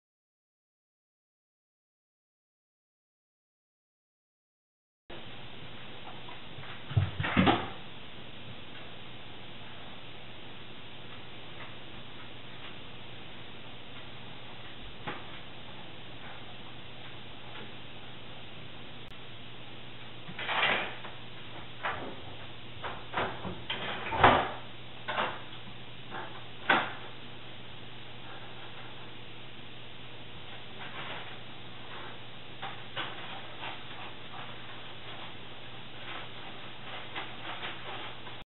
Rufus the Bad Dog Opens the Microwave
My dog Rufus smelled something he liked in the microwave and opens the door with his teeth to get it.